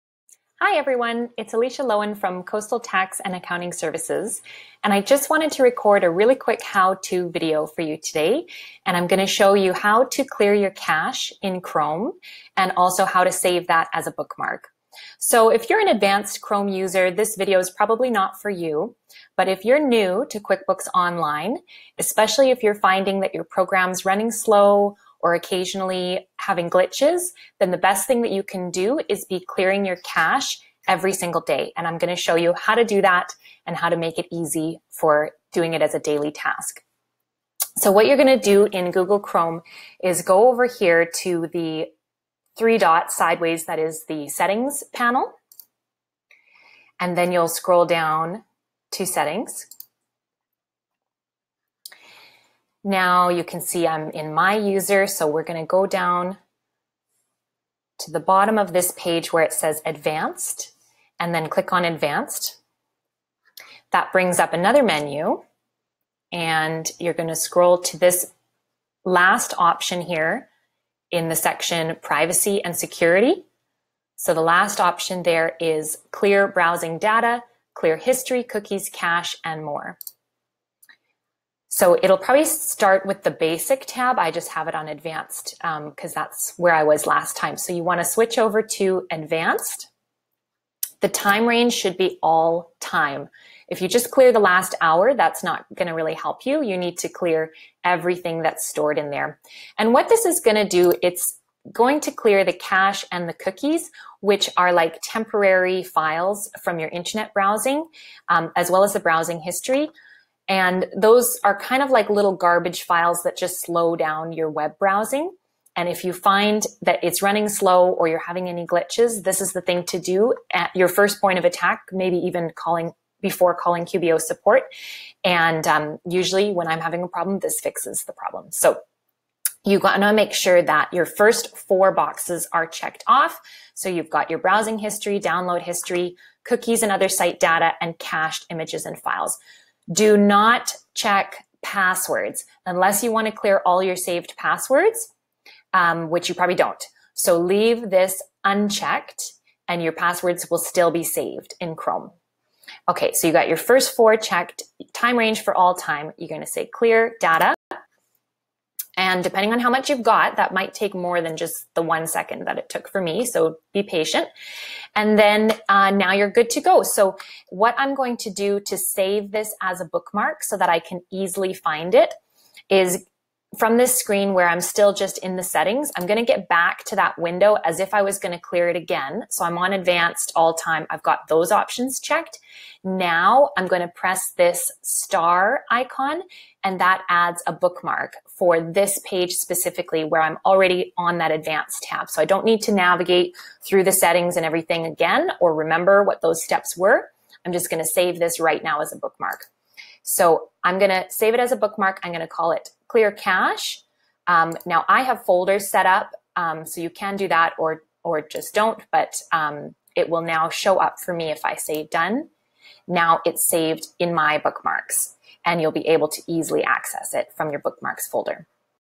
How To Clear Cache and Save As Bookmark
How to Clear Cache in Google Chrome and save as a Bookmark for future use